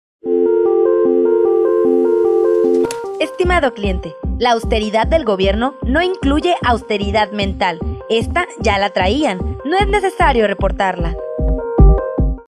Costalazo Político - Lo que Natura no da, Salamanca no Presta
Esta semana, nuestra operadora se asincera sobre la austeridad de Gobierno. ¡Entérate!